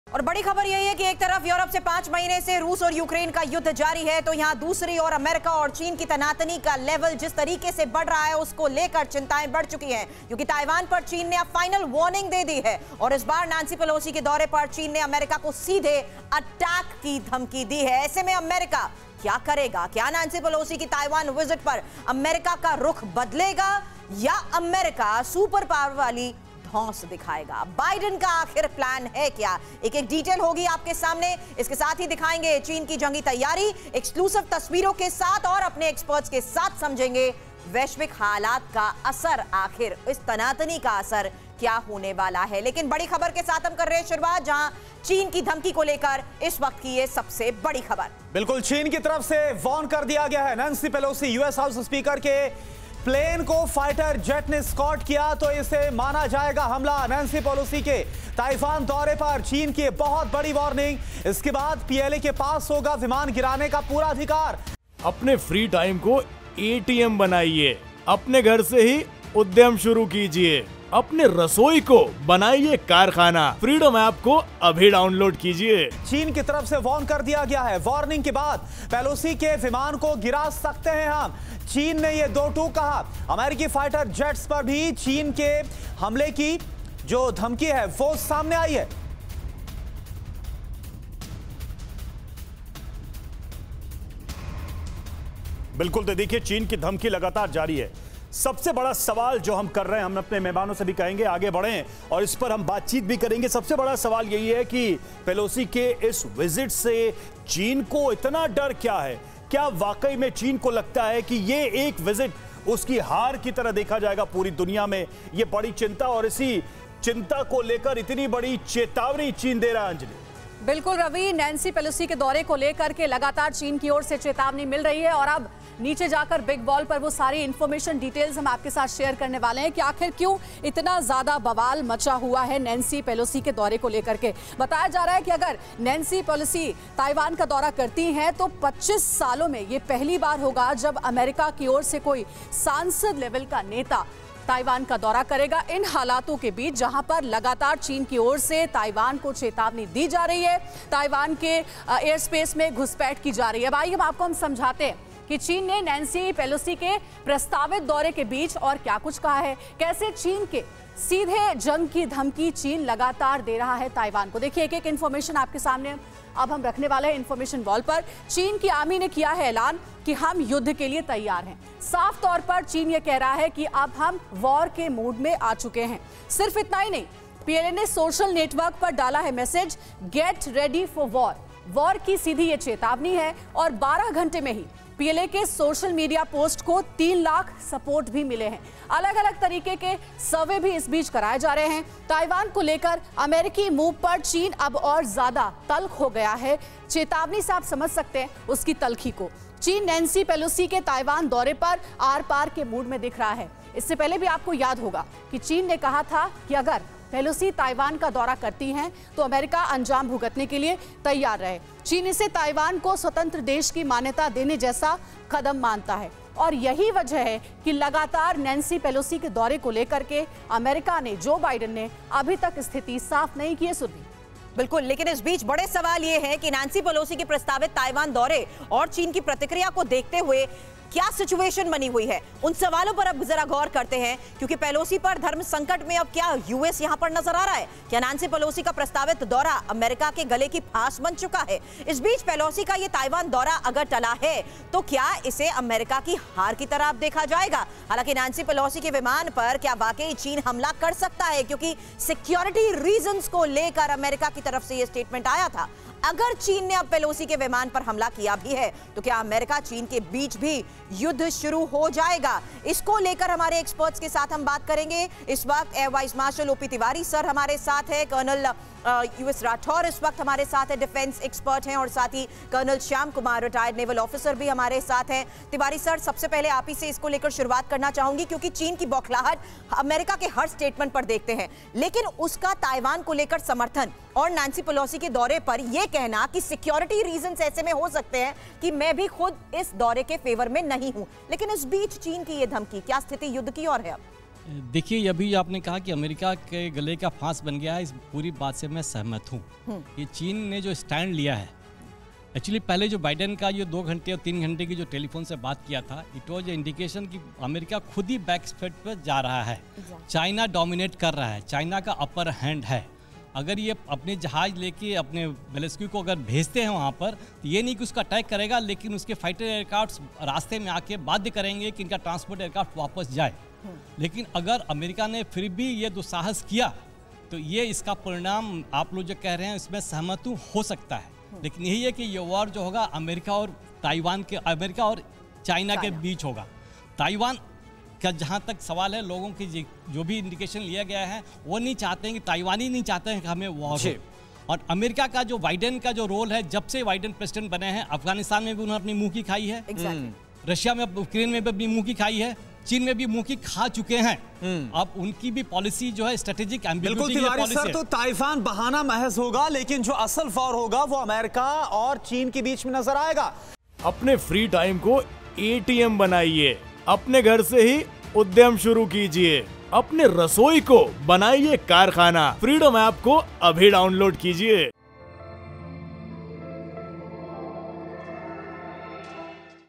Taiwan को लेकर America की मूव पर China तल्ख, China की आर्मी ने किया ऐलान- हम युद्ध के लिए तैयार हैं
अपने घर  से आसानी से एक लाभदायक व्यवसाय शुरू करें। Indian Money का Ffreedom app डाउनलोड करें और पेशेवरों से सीखें।

आइए देखते हैं कि चीन ने नैंसी पेलोसी के प्रस्तावित दौरे के बीच और क्या कुछ कहा है? कैसे चीन की सीधे जंग की धमकी दे डाली है?